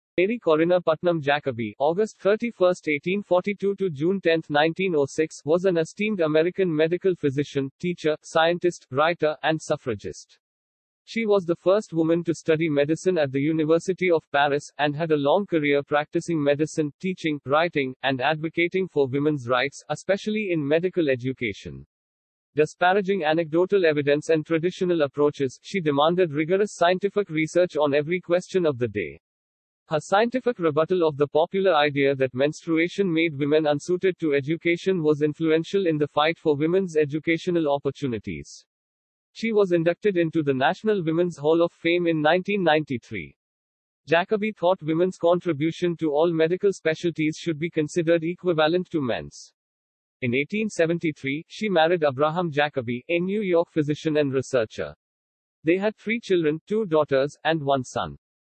Tribute to Mary Putnam Jacobi an esteemed American medical physician, teacher, scientist, writer
A Genuine Method to Generate Thousands of Dollars using FREE YouTube Videos Mary Corinna Putnam Jacobi (August 31, 1842 – June 10, 1906) was an esteemed American medical physician, teacher, scientist, writer, and suffragist. She was the first woman to study medicine at the University of Paris and had a long career practicing medicine, teaching, writing, and advocating for women's rights, especially in medical education.[2] Disparaging anecdotal evidence and traditional approaches, she demanded rigorous scientific research on every question of the day. Her scientific rebuttal of the popular idea that menstruation made women unsuited to education was influential in the fight for women's educational opportunities. She was inducted into the National Women's Hall of Fame in 1993. Jacobi thought women's contribution to all medical specialties should be considered equivalent to men's. In 1873, she married Abraham Jacobi, a New York physician, and researcher, nowadays often referred to as the "father of American pediatrics." They had three children, two daughters, and one son. youtubevideos.youtubemusic.youtubechannel.video.